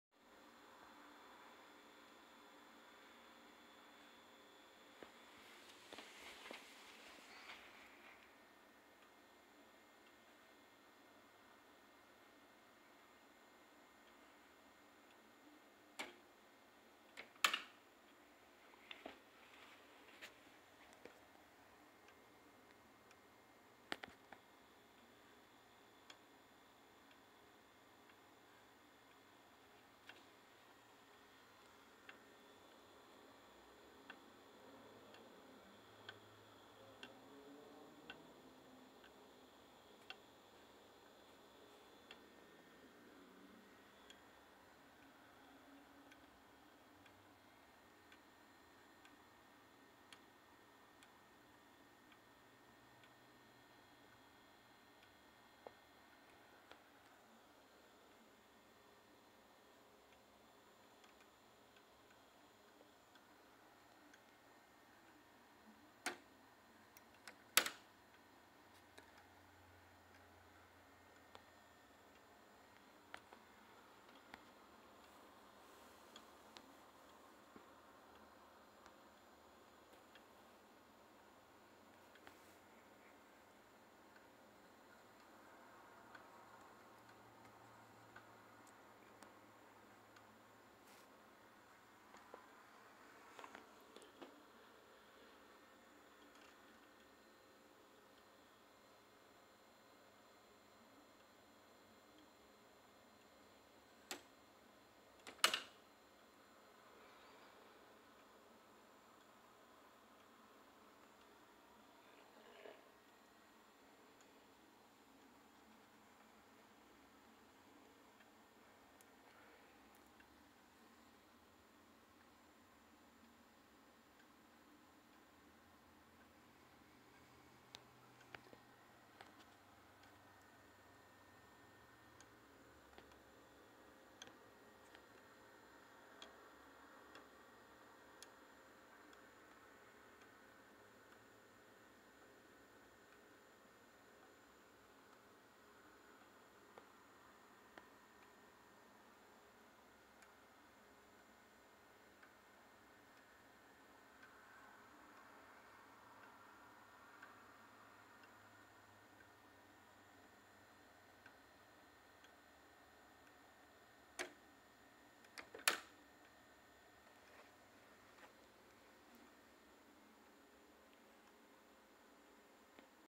My Nonus wooden model clock
Built from kit sold by woodentimes.com. Stained frame and clock face with black graphite coated movement.